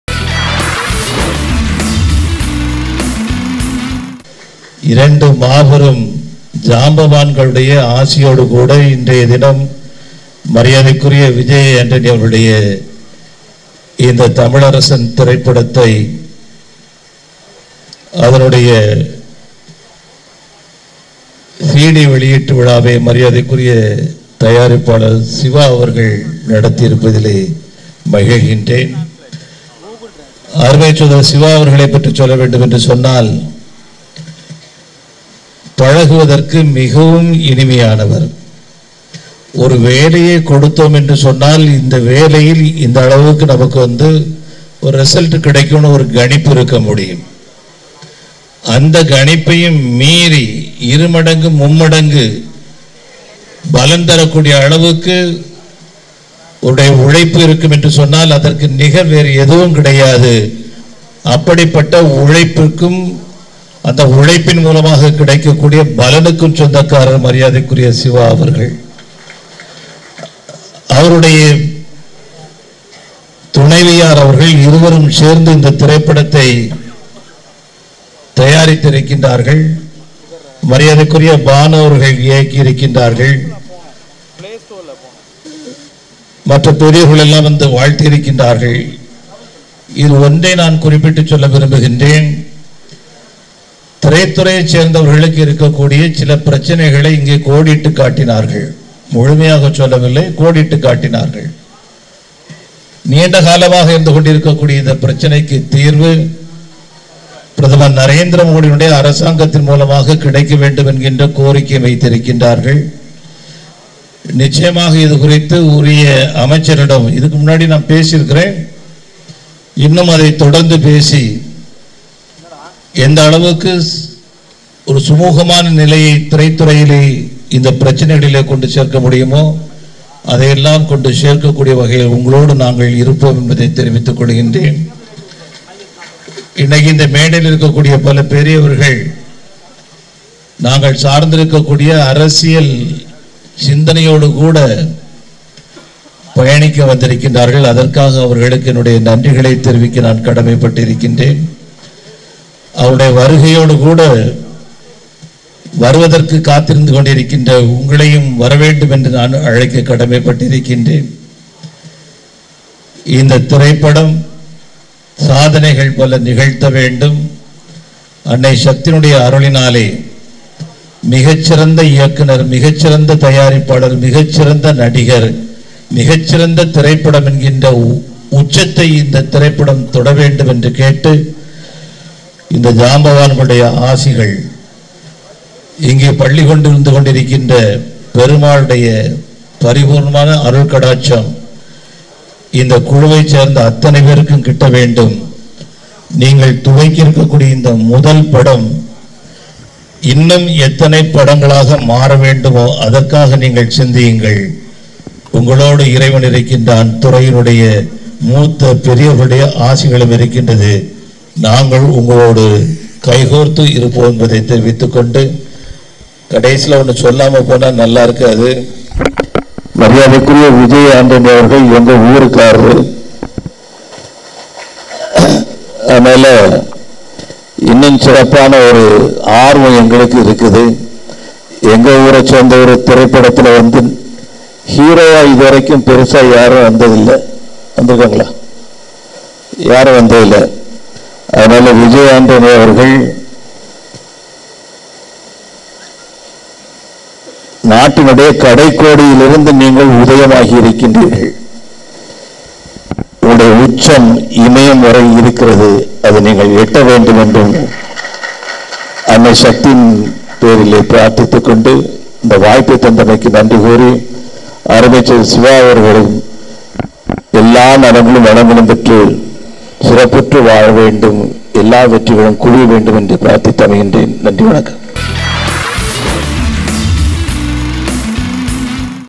திரைப்படத்துறை பிரச்னைகள் தீர்க்கப்படும் | Ponradha krishnan Speech at Tamilarasan Audio Launch
Ponradha krishnan at Speech at Tamilarasan Audio Launch

Vijay Antony |  Remya Nambeesan | Ilaiyaraaja | Babu Yogeswaran | Radharavi